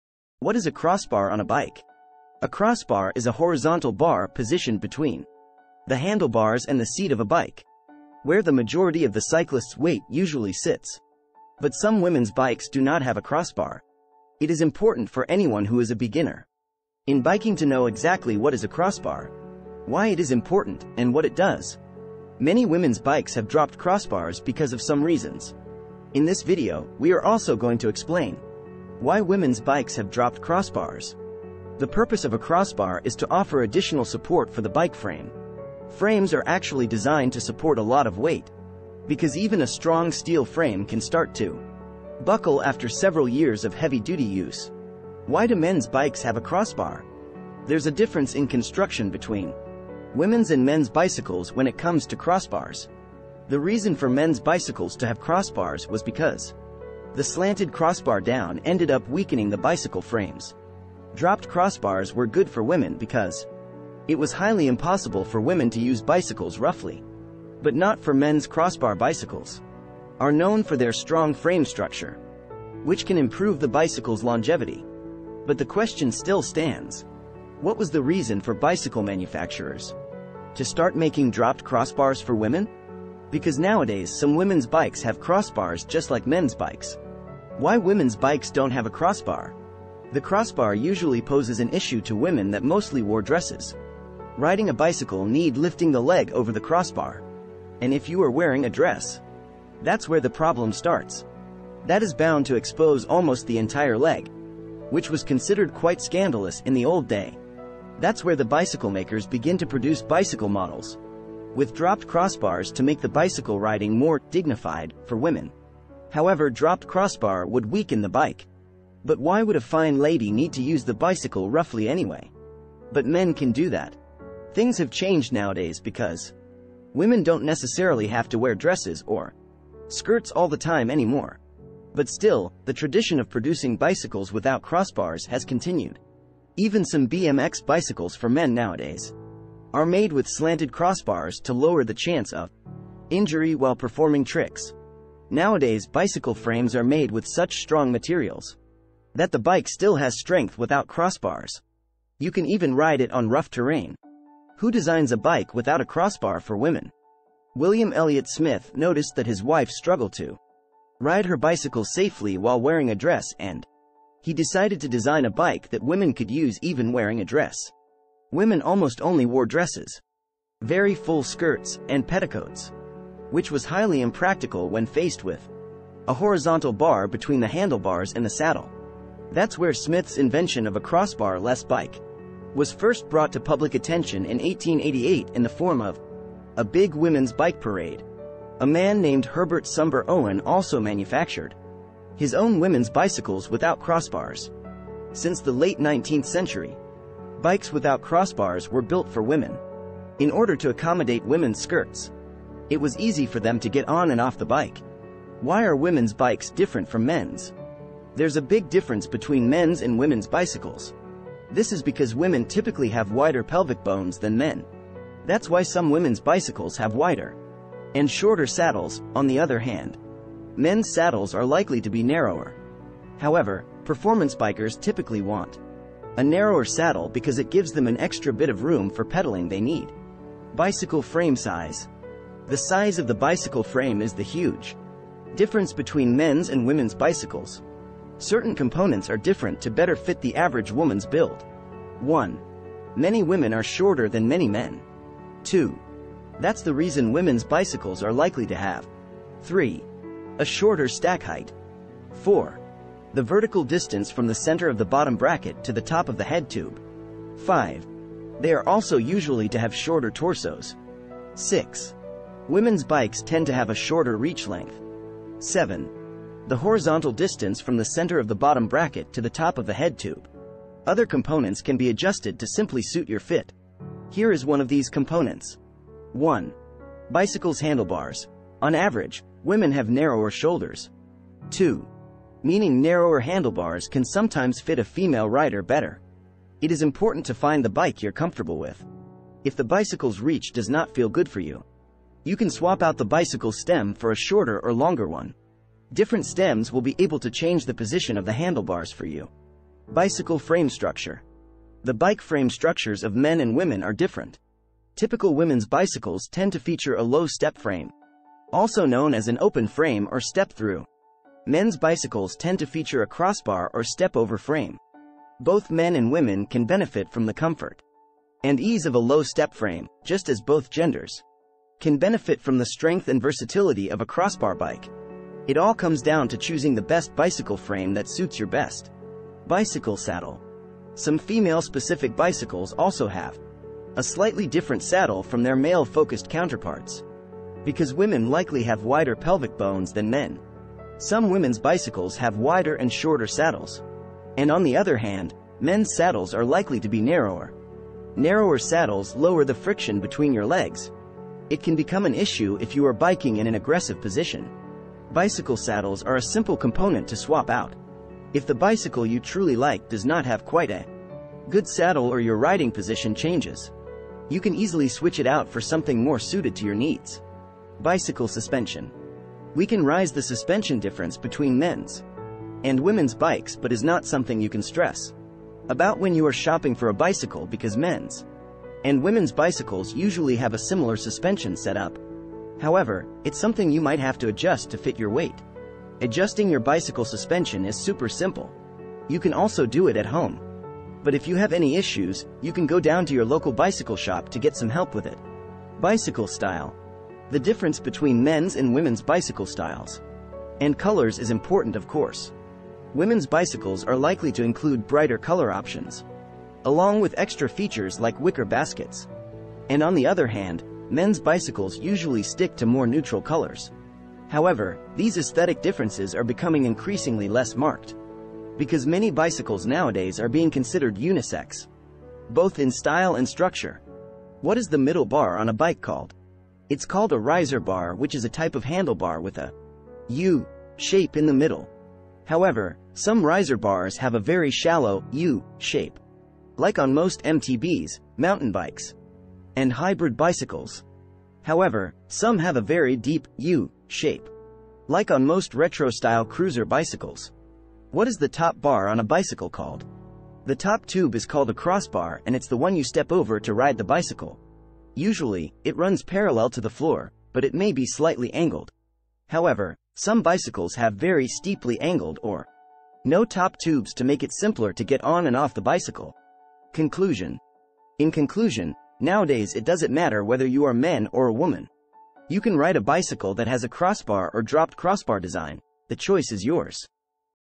What is a Crossbar on a Bike? What You Need To Know
A crossbar is a horizontal bar positioned between the handlebars and the seat of a bike, where the majority of the cyclist’s weight usually sits. But some women's bikes do not have a crossbar. It is important for anyone who is a beginner in biking to know exactly what is a crossbar, why it is important, and what it does?   Many women's bikes have dropped crossbars because of some reasons, in this article, we are also going to explain why women's bikes have dropped crossbars The purpose of a crossbar is to offer additional support for the bike frame. Frames are actually designed to support a lot of weight because even a strong steel frame can start to buckle after several years of heavy-duty use.